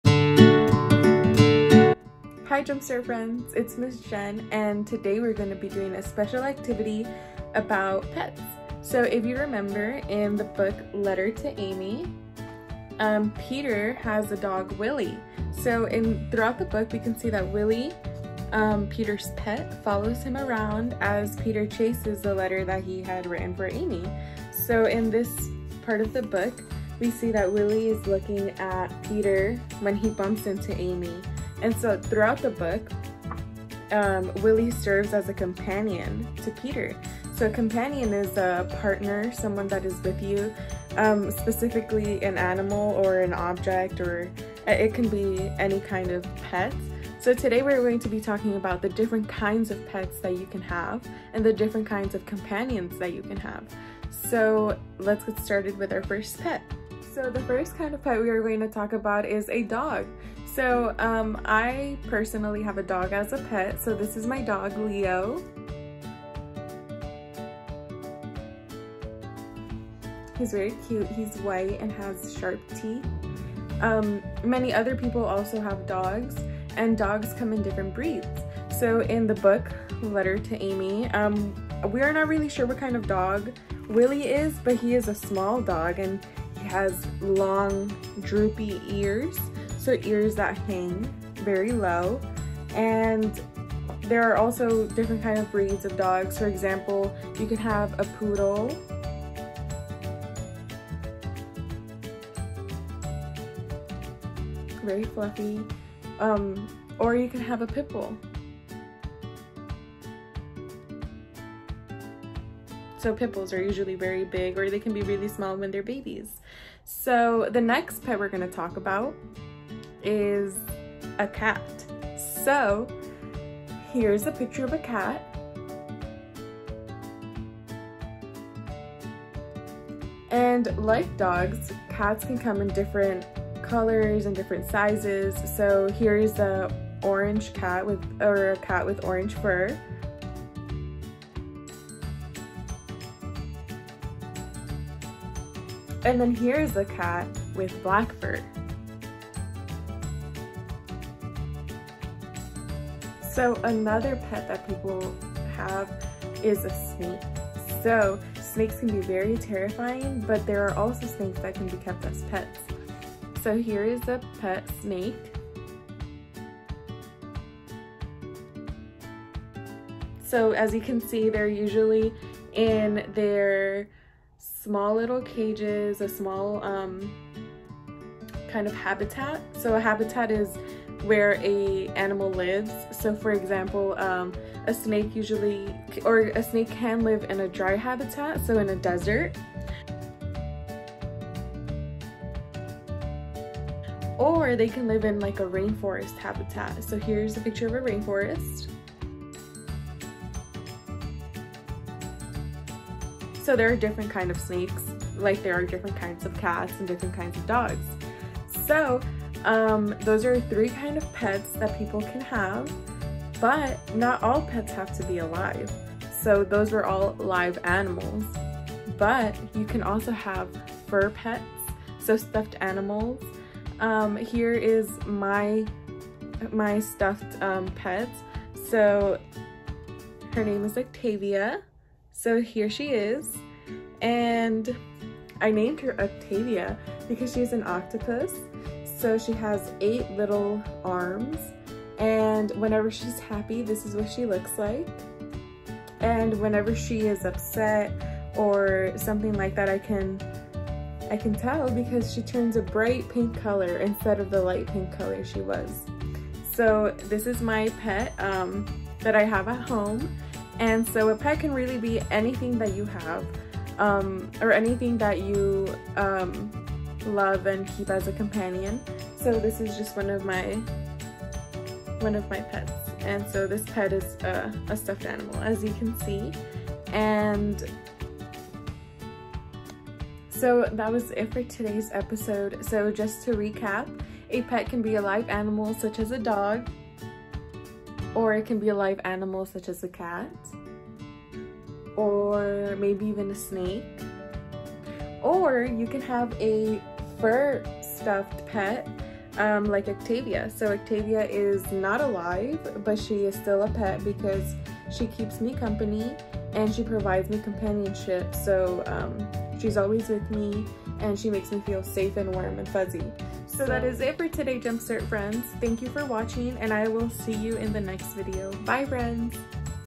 🐍🐶 Learn Along: Pets Are Friends Too!🐱🐙
Join Miss Jen in learning about all the different types of pets that can be our pets!